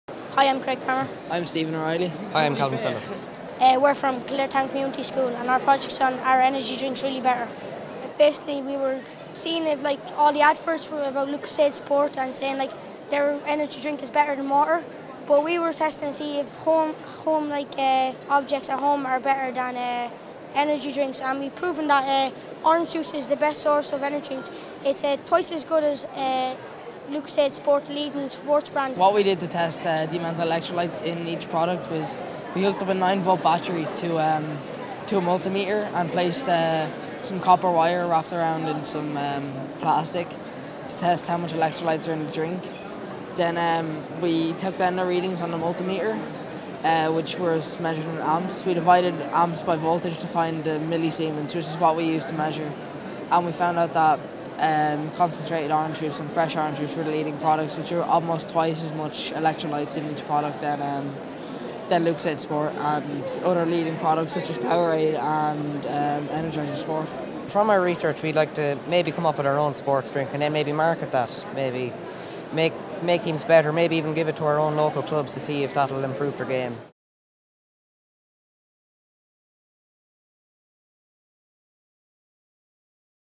Are Energy Drinks Really Better?
Students from Kildare Town Community School investigated if sports energy drinks really are better.